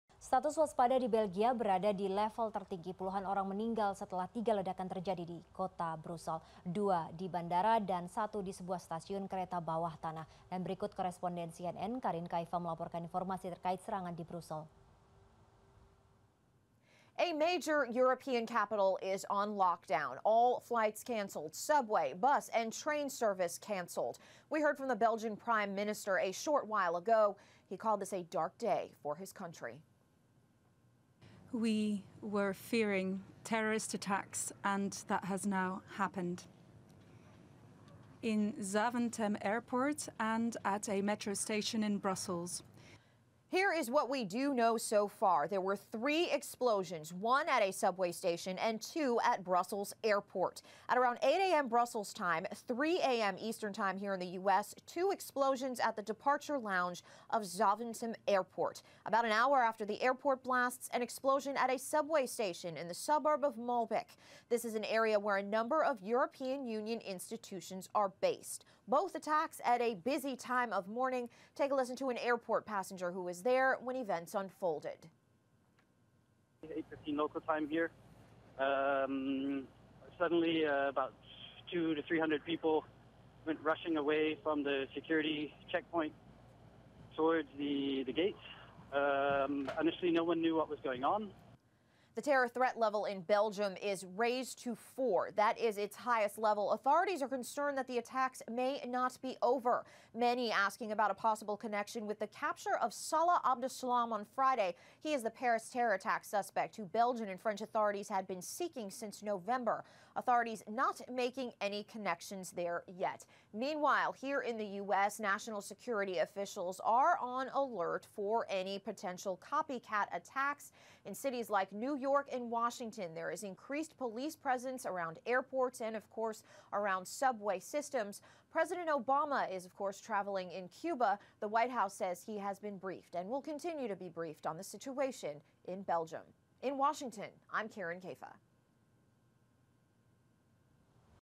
3 Ledakan di Belgia Incar Jumlah Korban yang Banyak
Tiga Ledakan Guncang Belgia, 34 Orang Dilaporkan Tewas
Korban tewas akibat tiga ledakan yang mengoyak ibukota Belgia, Brussels, Selasa (22/3) pagi waktu setempat bertambah menjadi 34 orang dan menyebabkan sedikitnya 170 orang terluka.